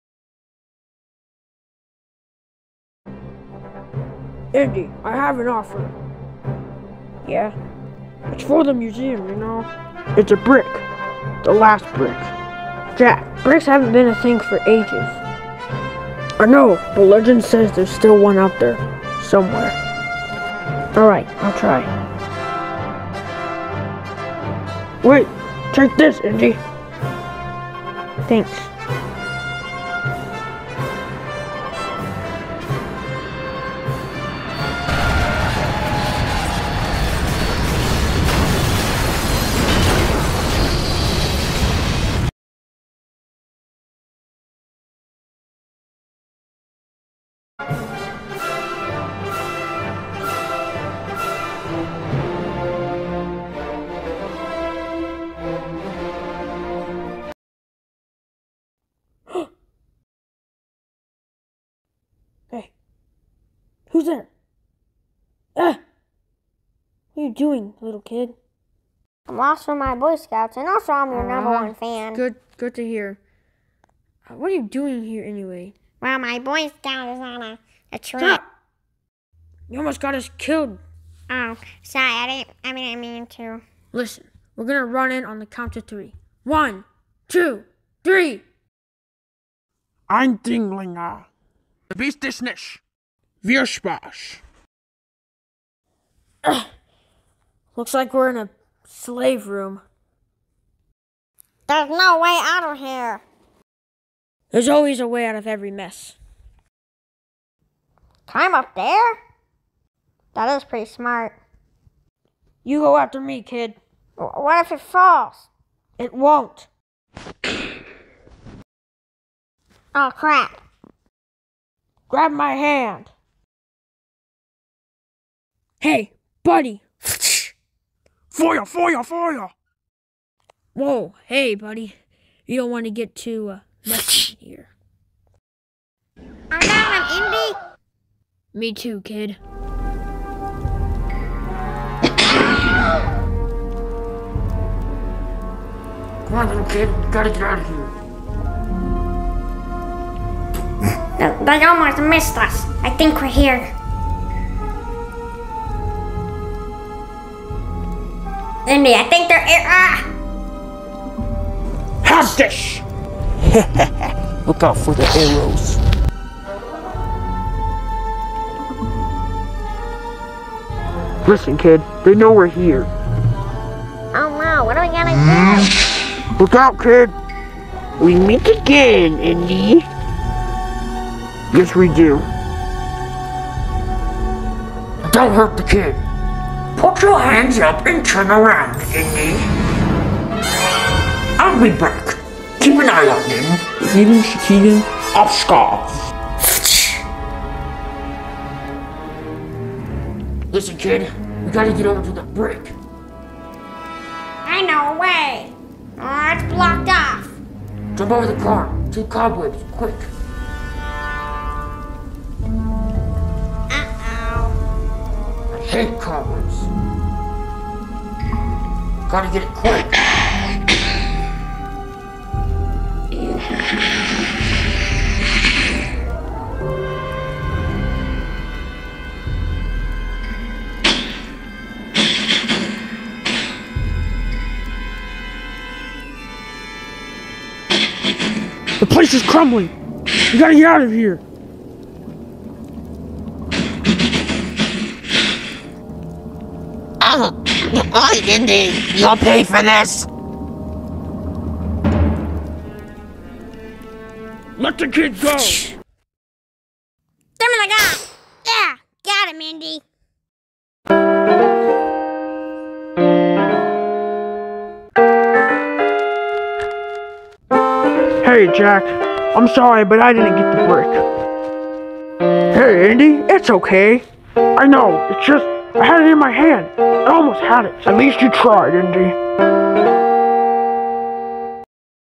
Indiana jones and the Last brick
Indiana jones trys to get the last brick from the brick temple but the Germans get in his way.